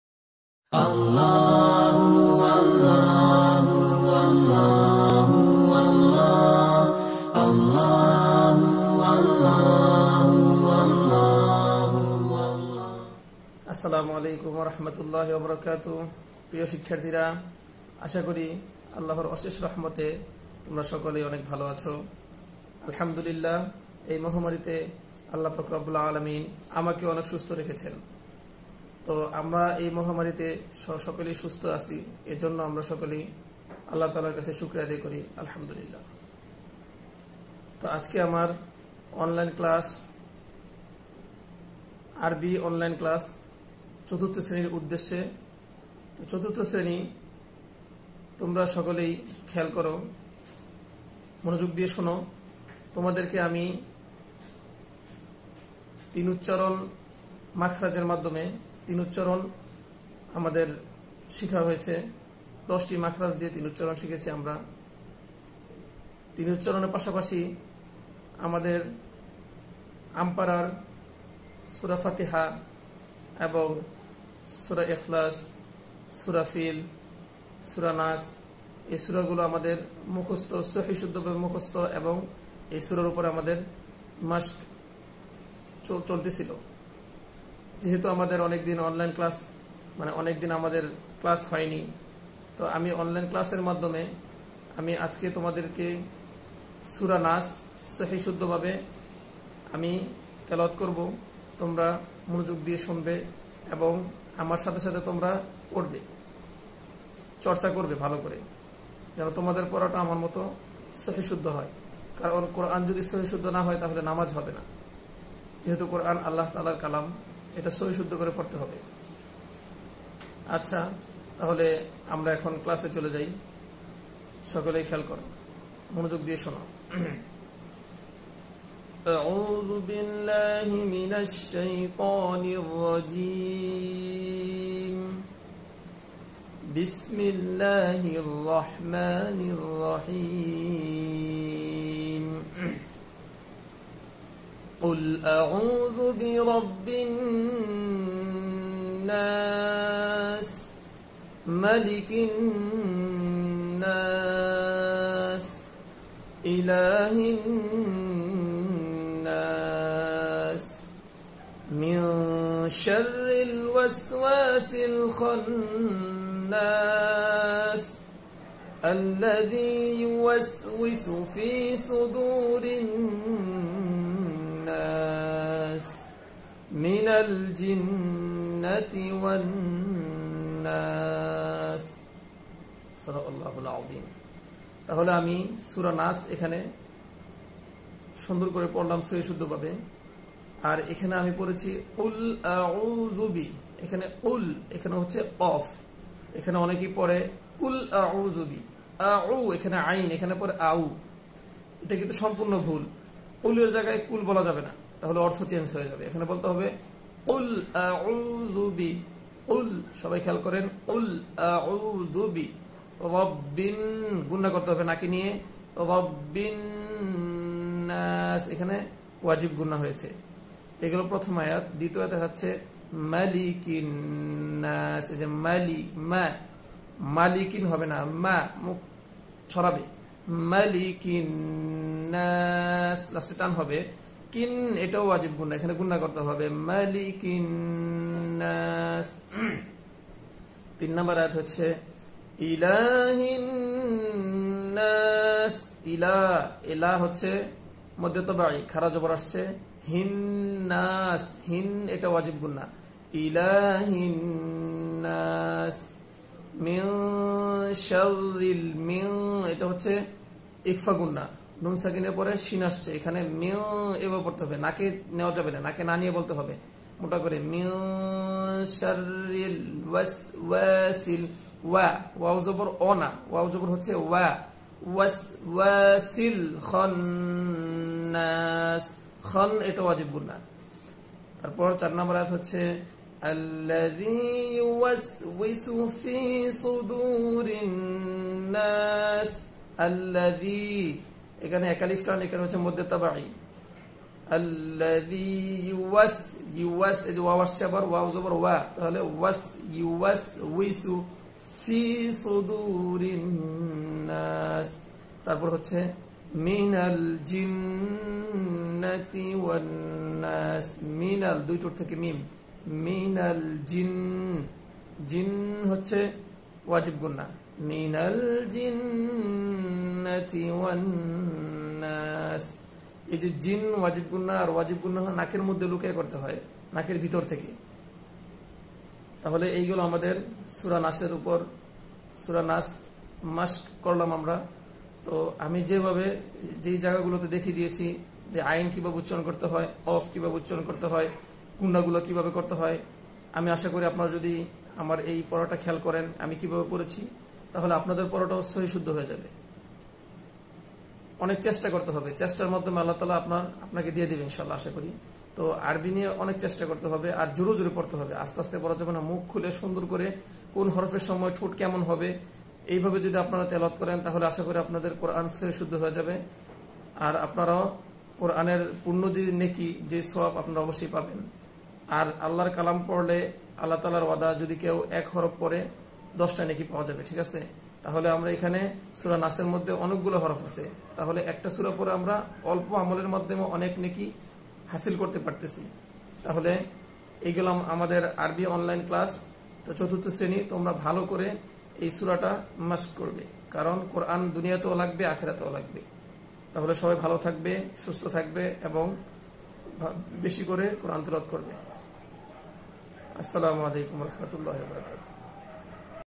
শ্রেণি: ৪র্থ (আরবি) | অনলাইন ক্লাস | বাংলা ভার্সন | AFSC 2020
অনলাইন ক্লাস (বাংলা ভার্সন)
শ্রেণি: ৪র্থ
বিষয়: আরবি
শিক্ষক: এইচ.এম. ইমরান হোসেন